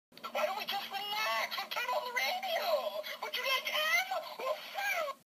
Would you like M OR FMMMMM?
I'm so sorry I had to draw him again ;w; XD also I'm working on a 320+ sub special (I'm almost done it) so I just wanted to upload this because it's frikin hilarious XD

(Also he's a fan of Maroon 5 so he got pretty peeved when it said F(udge) M(aroon 5) .3.)